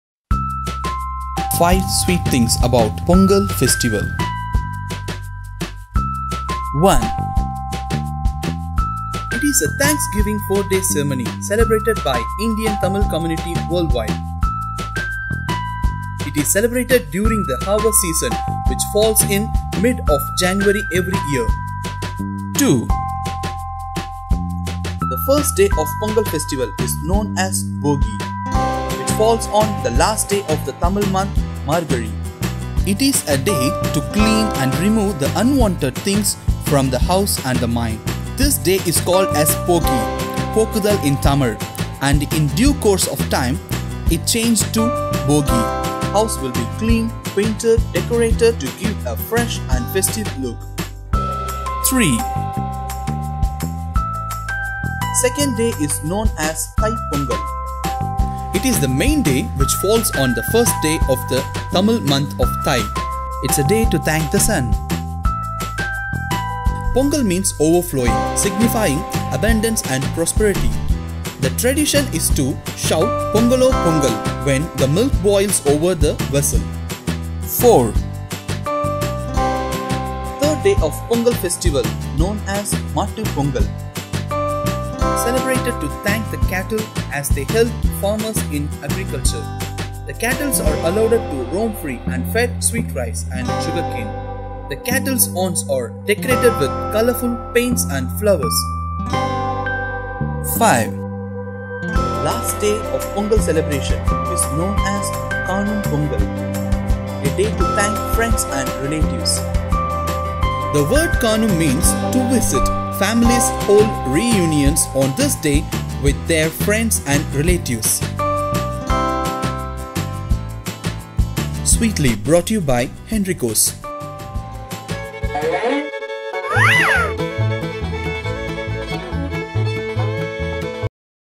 5 Sweet Things About Pongal Festival | ENRICO'S
Hello all!

Before we jump into our joyous ponggal celebrations for 2019, here’s 5 SWEET facts about Pongal that we want to share with all of you!

A very Happy & Sweet Pongal folks!

With Love, Enrico’s